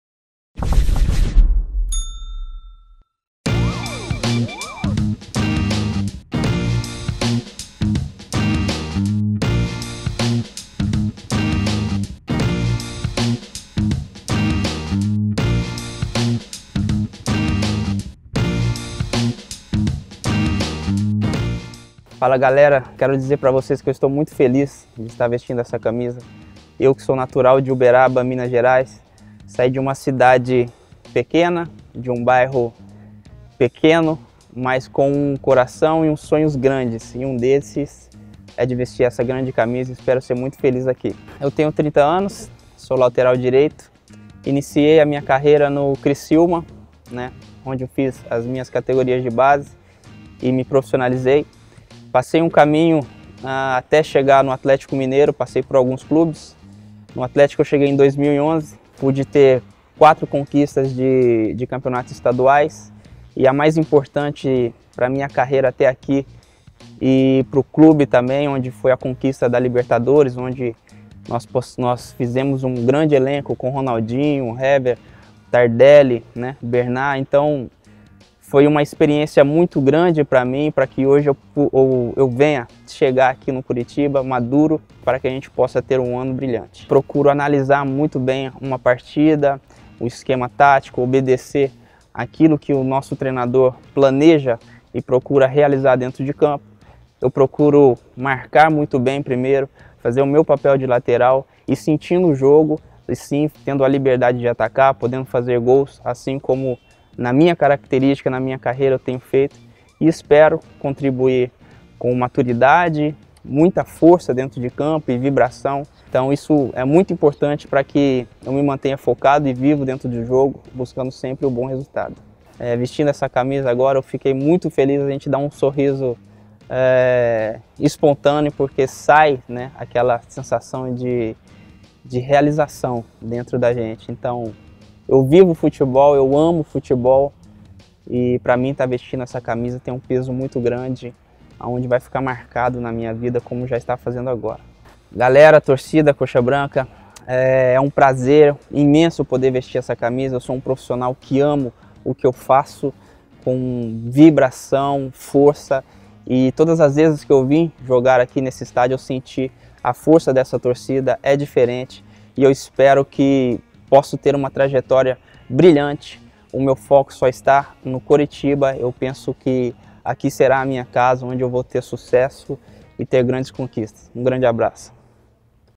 Apresentação Carlos César
Projetando o Brasileiro, Coxa anuncia lateral direto, que chega com contrato até o final da temporada